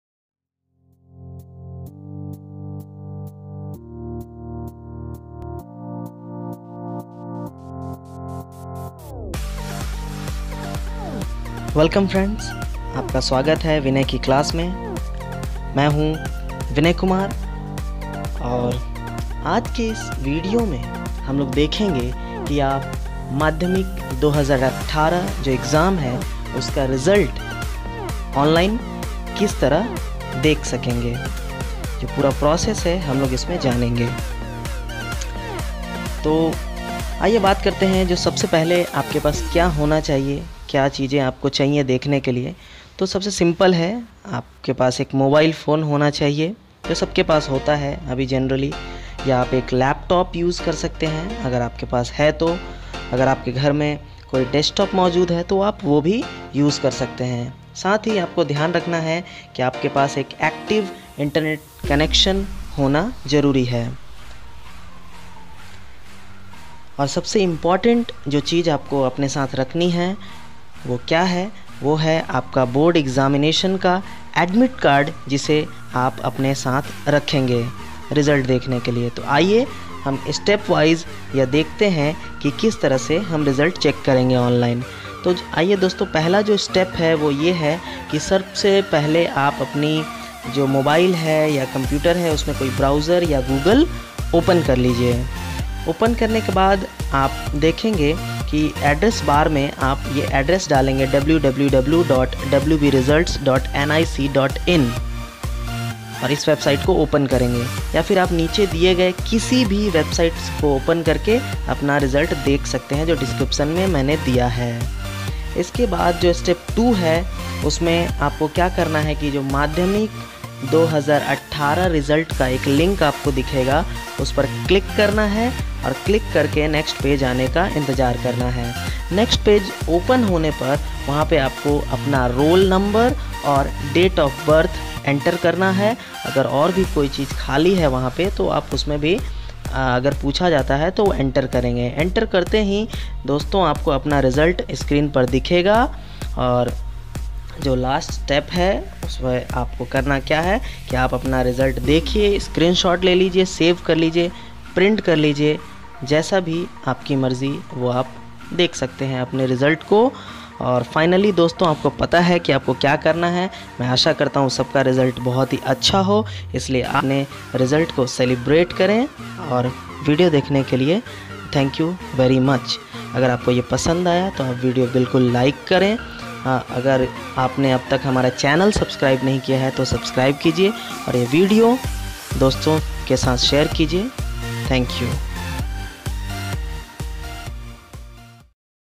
How to check Board Exam Result 2018 | Madhyamik Result | Learn in 3 minutes | Vinay Kumar | ECC
The West Bengal Board of Secondary Education is going to announce the result of Madhyamik examination 2018 in June 2018 . The examination was organised in the month of March, 2018. The result will be published on the official websites
The results can be viewed on the following websites:

Or students can simply search on GOOGLE for "WEST BENGAL MADHYAMIK RESULT 2018" on the date of announcement. Click any link from the Search result to get your result.

How to check your result?

1. First go to any of the above website.

2. Now click on the link Madhyamik Pariksha Result 2018

3. Enter your Roll Number and Date of birth.

4. Thats done! You can View and Print your result.